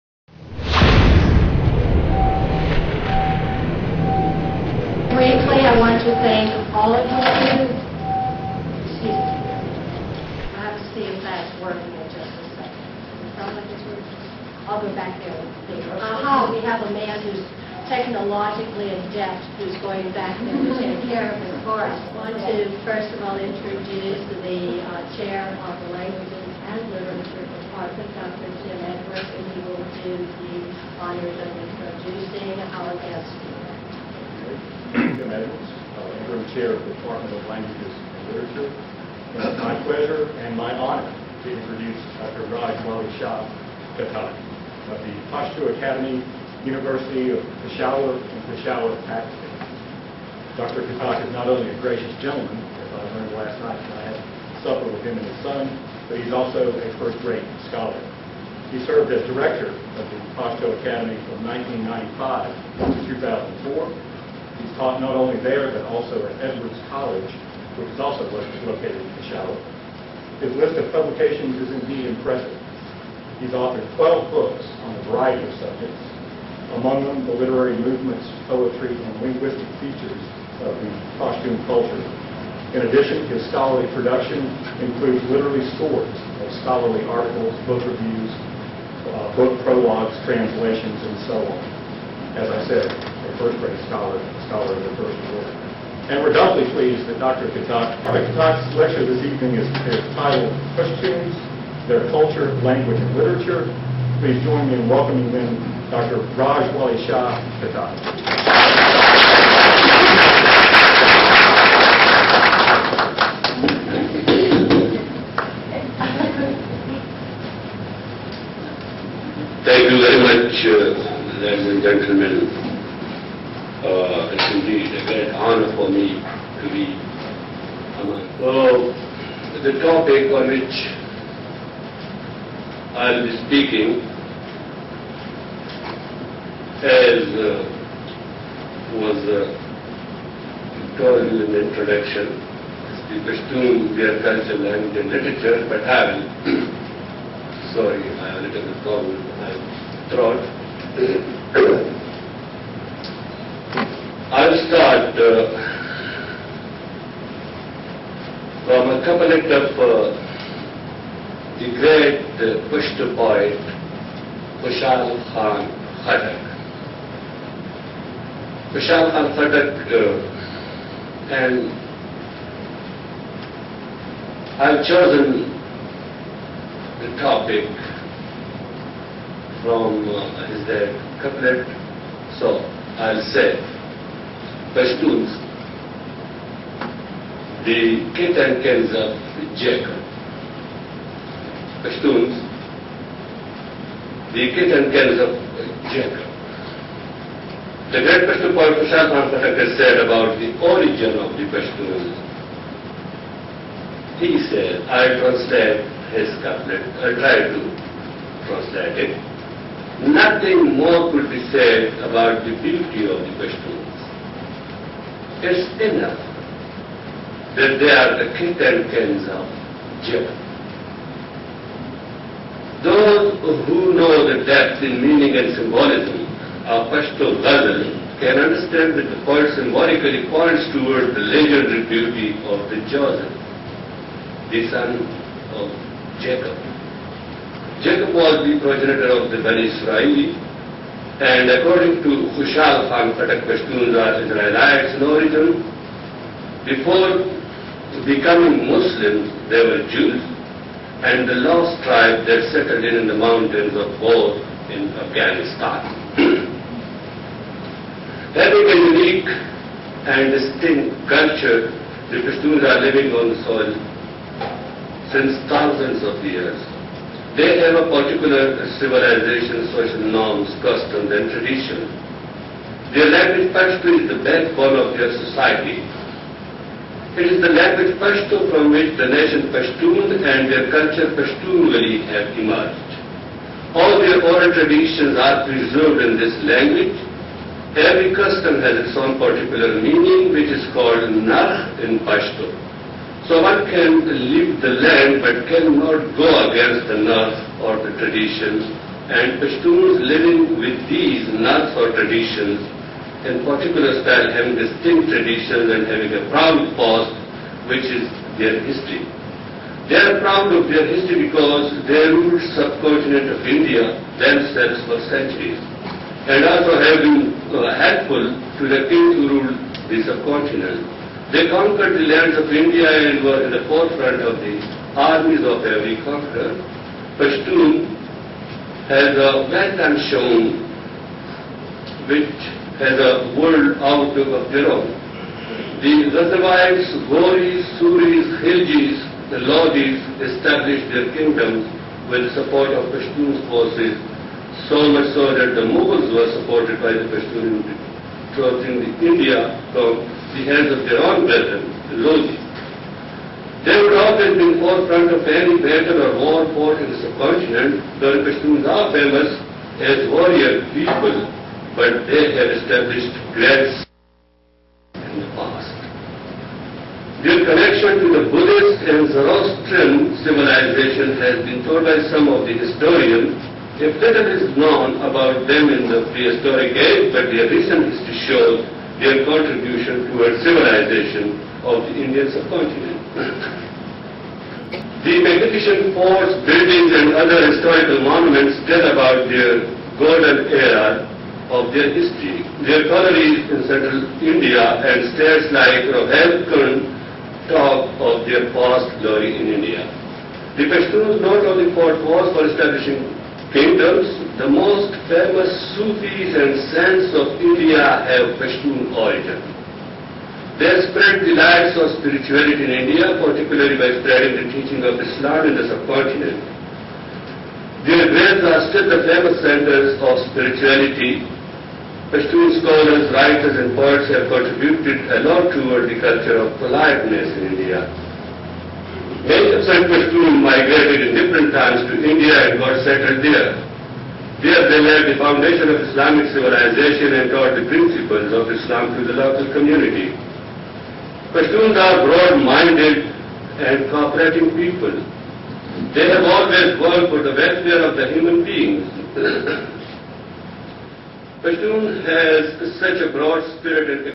About the origin of Pashtuns part 01
In this video, i want to let our pashtun people know about the one theory of pashtuns discussed by Dr. Raj Wali Shah Khattak